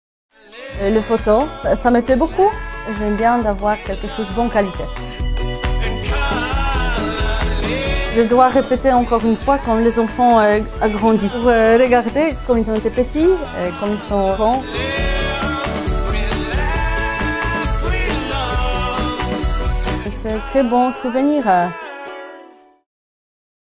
Thierry Seguin Photographie (Spot 30s)
Une séance photo de famille dans un studio de shooting photo famille. Le Photographe de mariage réalise également des photographies de portraits. Photographe de femme enceinte, il confectionne votre album photo grossesse, mais aussi un shooting photo de bébé, qu'il soit nouveau né ou un peu plus vieux.